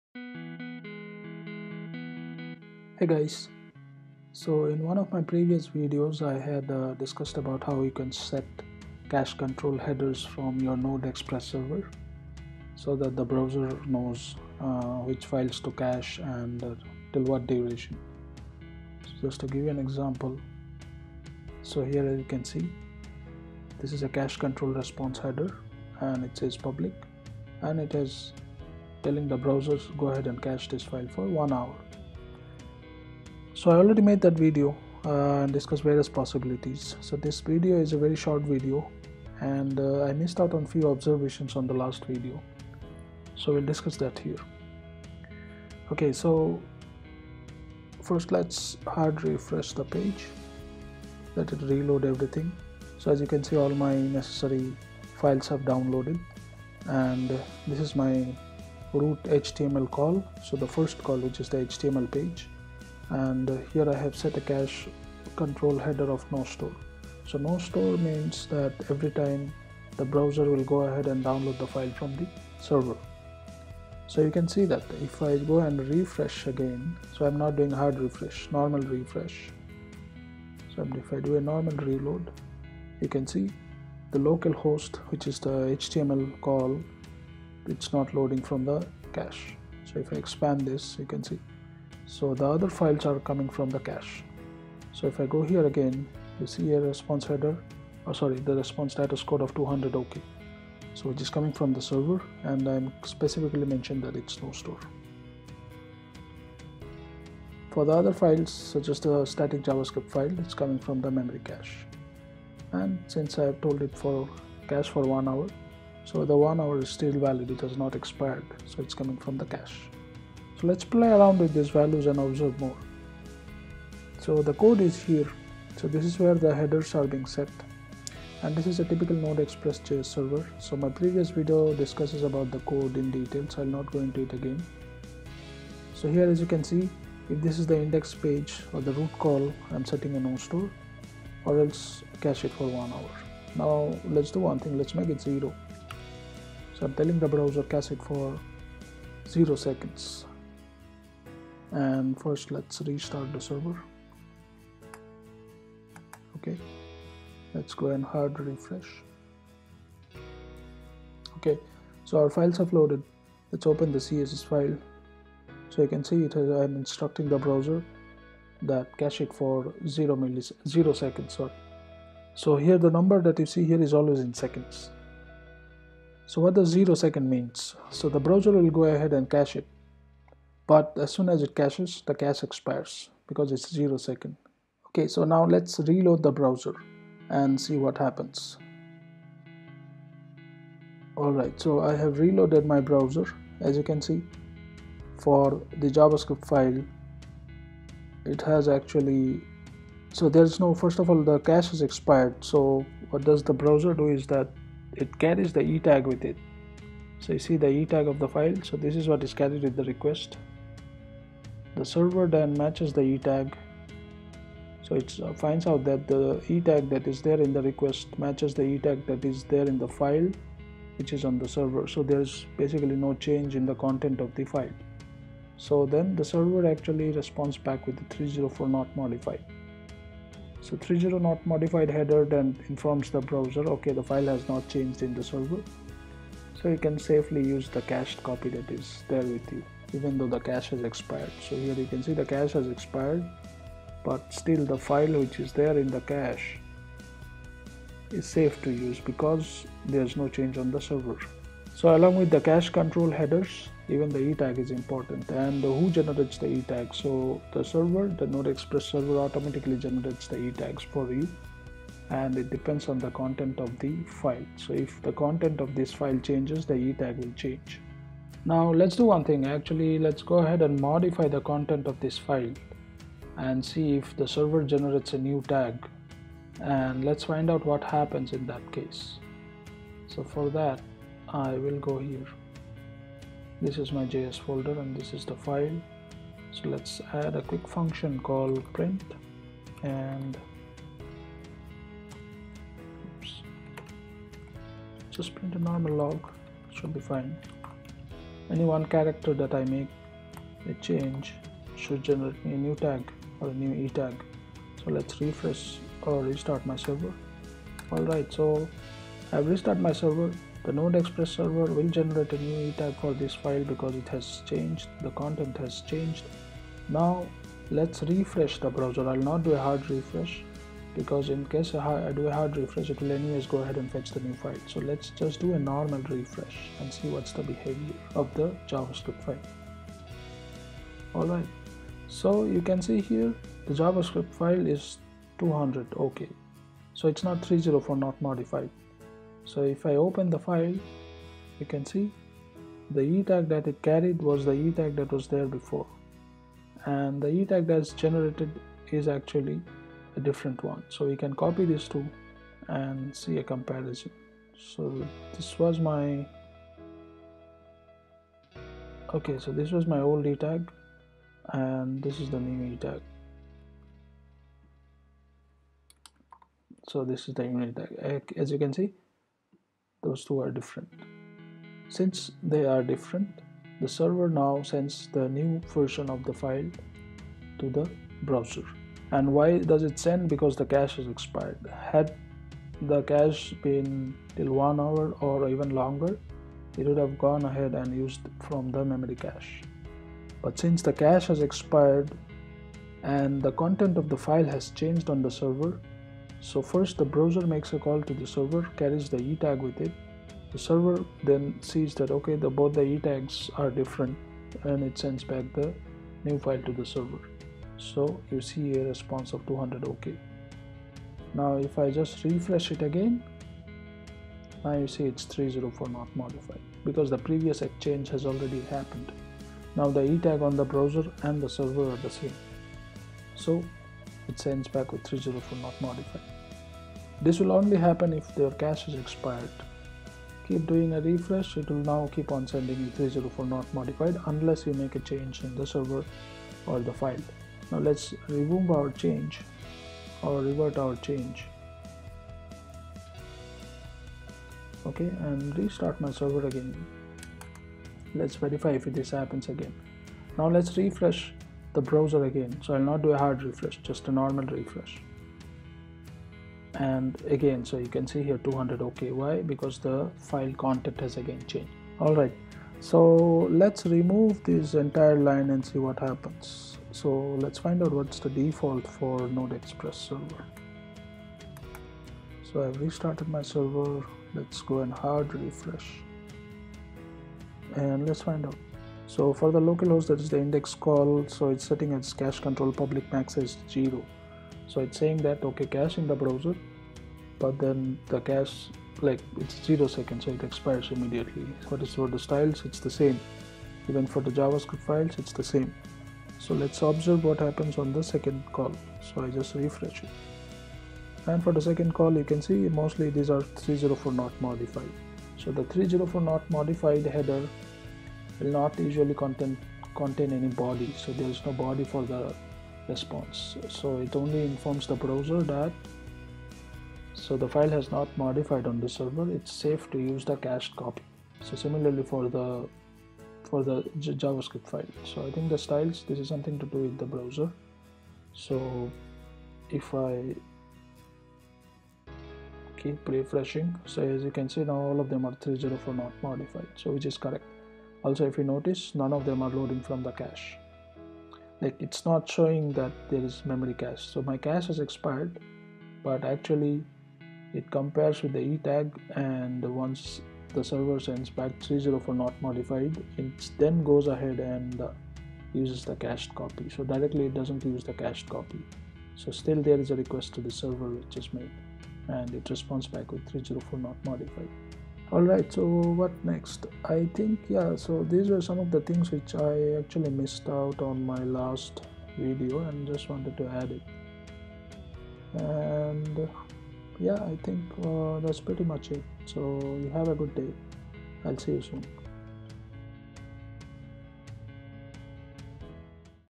Caching Static Files with Node Express.js server. Inner Details explained
Some questions we answer in this video:
Cache-Control: max-age
ETag
How the web browser caches static content?
How you can set cache headers in response?
Resource Prioritization in Web Browsers?
What is Memory Cache?
What is Disk Cache?
Caching static files in the Node ExpressJS server.

Link to the previous video (watch this for a detailed explanation with code example)

Node ExpressJS server can serve static files using the express.static middleware. We can pass options to this middleware and set Cache-Control headers in the response. The browser will then go ahead and cache the files.
Examples of Cache-Control headers
Cache-Control: public, max-age: 3600 (cache both on browser and CDN)
Cache-Control: private, max-age: 3600 (cache only on the browser)
Cache-Control: no-store (do not cache)
Cache-Control: no-cache, max-age: 3600 (cache the file, next time ask the server if new version available, or else use the cached file)

We also discussed on E-Tags and How web browsers prioritize resource downloads. How web browser caches content. The first call to the server for HTML is not cached by the browser, but CDN’s might cache it. This can be a problem.